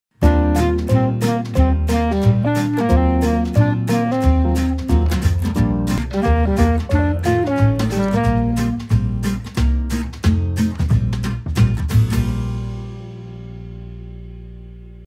Galtee Crumbed Ham, 360g, only £2.25.
Getting paid in january is a big deal, but its not a MEGA deal! Galtee Crumbed Ham, 360g, only £2.25. Now that’s value when you need it most. Deal availble from 23-29th Jan.